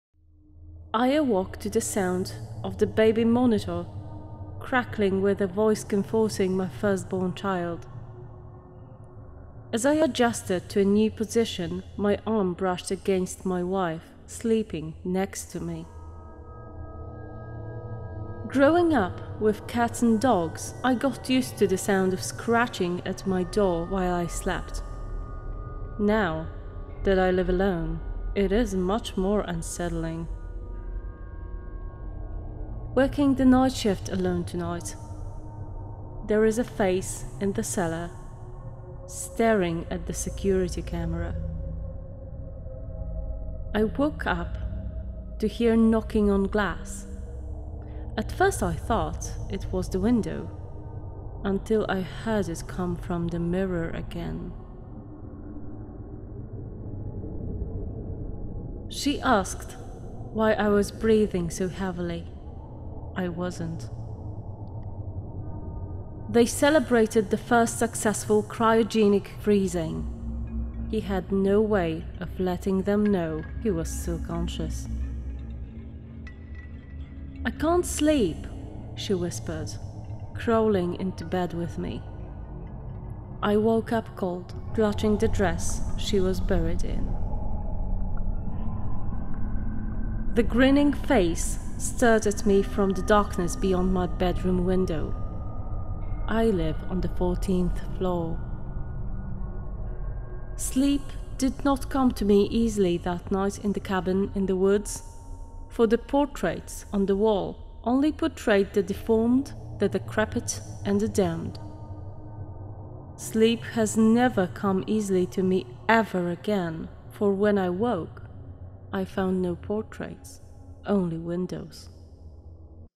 TOP 10 BEST AND MOST SCARY SHORT CREEPYPASTAS FOUND IN THE INTERNET
Do you like scary stories? Creepy, and bone-chilling tales which will make you sleepless? You're in right place, enjoy!